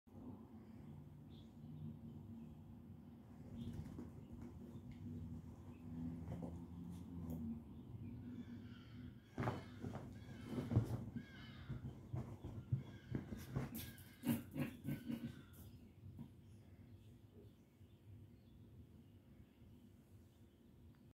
Coco & Luna Yorkie Sisters
Two Yorkie sisters, playing together